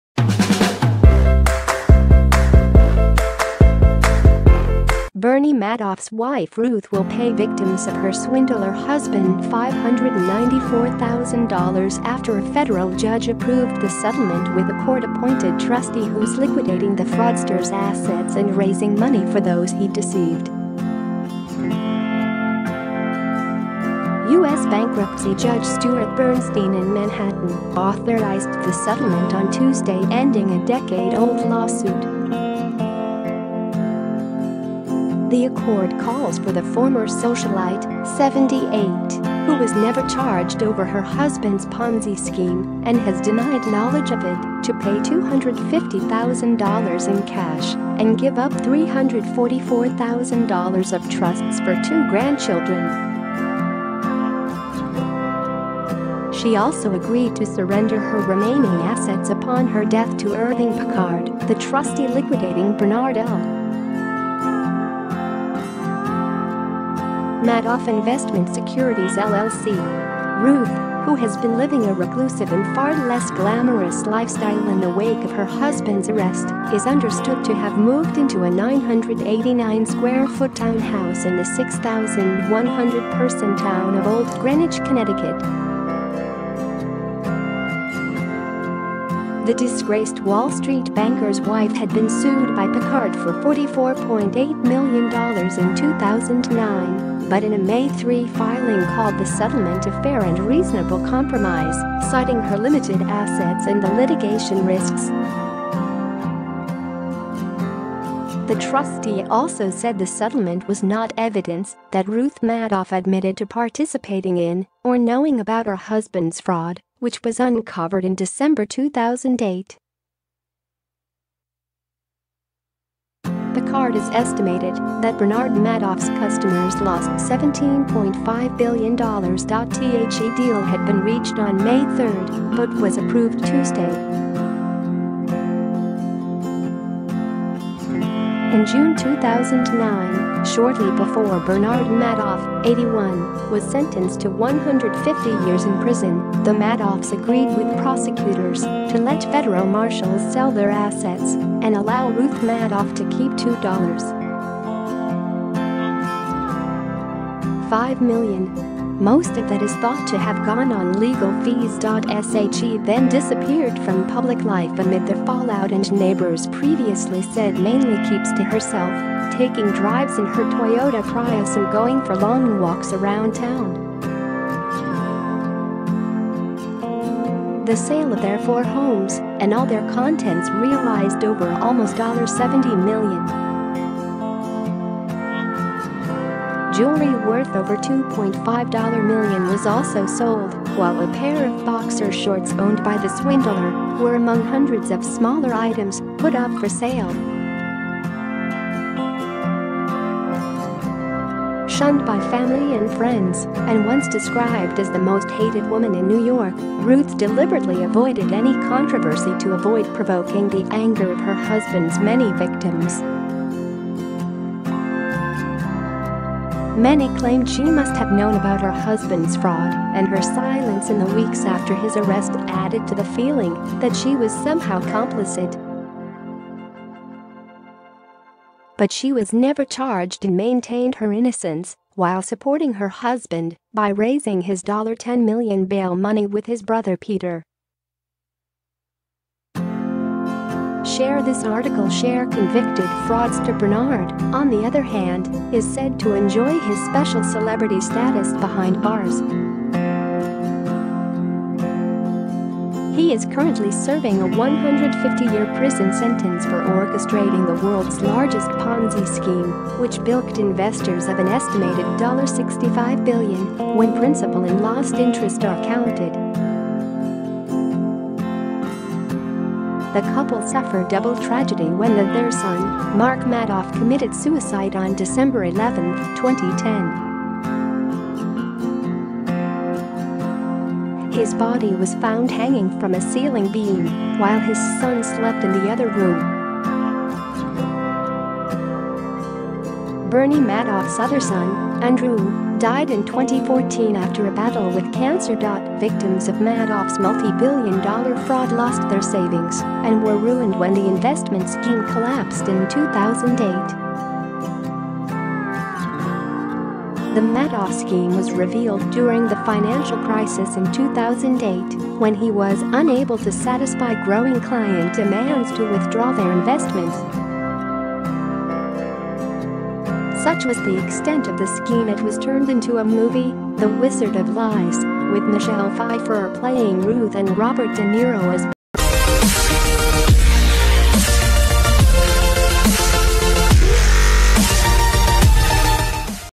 Bernie Madoff's wife Ruth will pay her husband's victims $594,000
Bernie Madoff's wife Ruth will pay victims of her swindler husband $594,000 after a federal judge approved the settlement with a court-appointed trustee who's liquidating the fraudster's assets and raising money for those he deceived. U.S. Bankruptcy Judge Stuart Bernstein in Manhattan authorized the settlement on Tuesday ending a decade-old lawsuit.The accord calls for the former socialite, 78, who was never charged over her husband's Ponzi scheme and has denied knowledge of it, to pay $250,000 in cash and give up $344,000 of trusts for two grandchildren.She also agreed to surrender her remaining assets upon her death to Irving Picard, the trustee liquidating Bernard L. Madoff Investment Securities LLC.  Ruth, who has been living a reclusive and far less glamorous lifestyle in the wake of her husband's arrest, is understood to have moved into a 989-square-foot townhouse in the 6,100-person town of Old Greenwich, Connecticut.The disgraced Wall Street banker's wife had been sued by Picard for $44.8 million in 2009, but in a May 3 filing called the settlement a 'fair and reasonable compromise,' citing her limited assets and the litigation risks.The trustee also said the settlement was not evidence that Ruth Madoff admitted to participating in or knowing about her husband's fraud, which was uncovered in December 2008.   Picard has estimated that Bernard Madoff's customers lost $17.5 billion.The deal had been reached on May 3 but was approved Tuesday. In June 2009, shortly before Bernard Madoff, 81, was sentenced to 150 years in prison, the Madoffs agreed with prosecutors to let federal marshals sell their assets, and allow Ruth Madoff to keep $2.5 million. Most of that is thought to have gone on legal fees.She then disappeared from public life amid the fallout and neighbors previously said mainly keeps to herself, taking drives in her Toyota Prius and going for long walks around town.  The sale of their four homes and all their contents realized over almost $70million. Jewelry worth over $2.5million was also sold, while a pair of boxer shorts owned by the swindler were among hundreds of smaller items put up for sale.Shunned by family and friends, and once described as the most hated woman in New York, Ruth deliberately avoided any controversy to avoid provoking the anger of her husband's many victims.Many claimed she must have known about her husband's fraud and her silence in the weeks after his arrest added to the feeling that she was somehow complicit.But she was never charged and maintained her innocence - while supporting her husband by raising his $10million bail money with his brother Peter.    Share this article   Share   Convicted fraudster Bernard, on the other hand, is said to enjoy his 'special celebrity' status behind bars.He is currently serving a 150-year prison sentence for orchestrating the world's largest Ponzi scheme, which bilked investors of an estimated $65billion when principal and lost interest are counted. The couple suffere